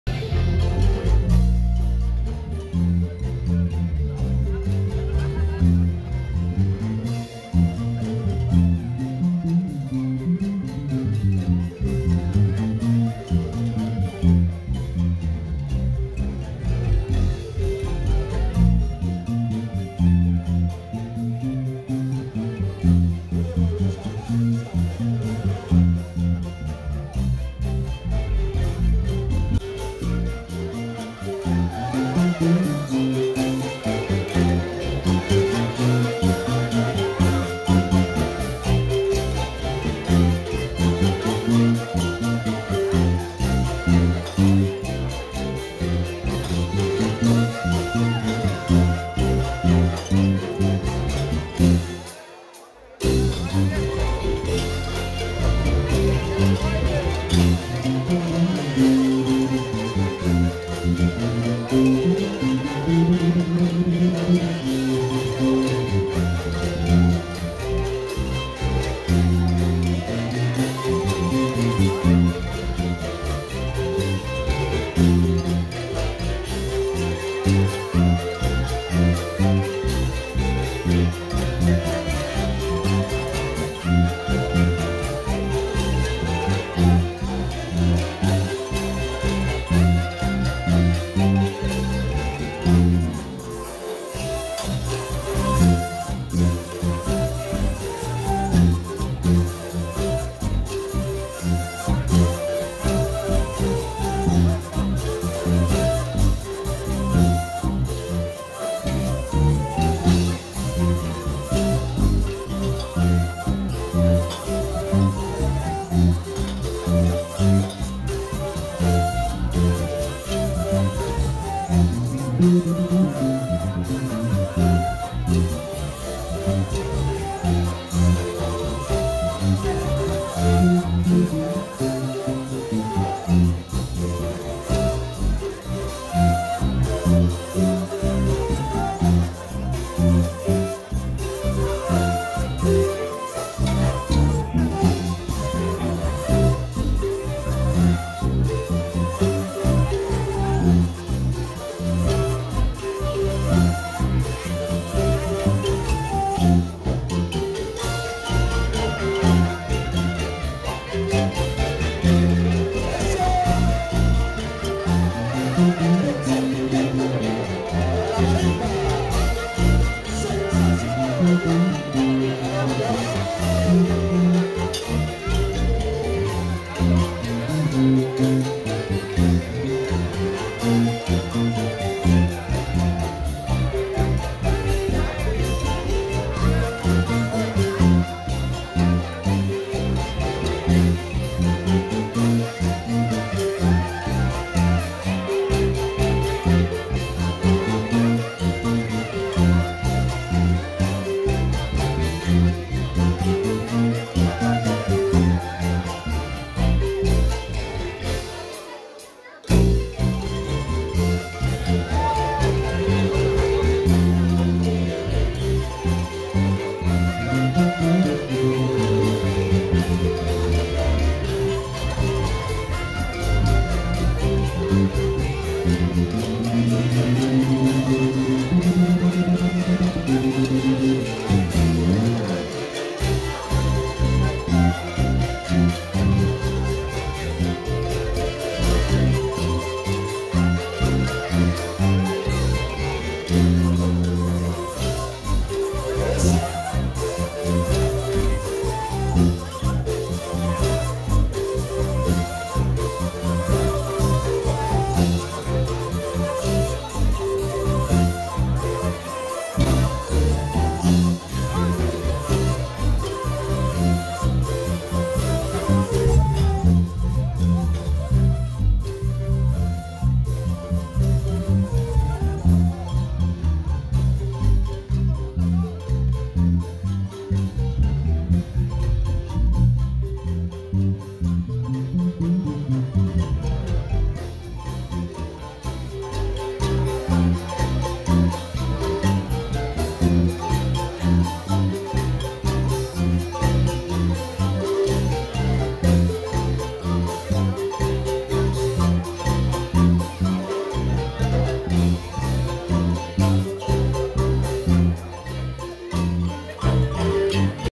7. Marimba Sensacion Jolom Konob' en Canton G.A | El dia de los Santos 2020
Baile Social en Canton, Georgia 2020 con la Marimba Sensacion Jolom Konob'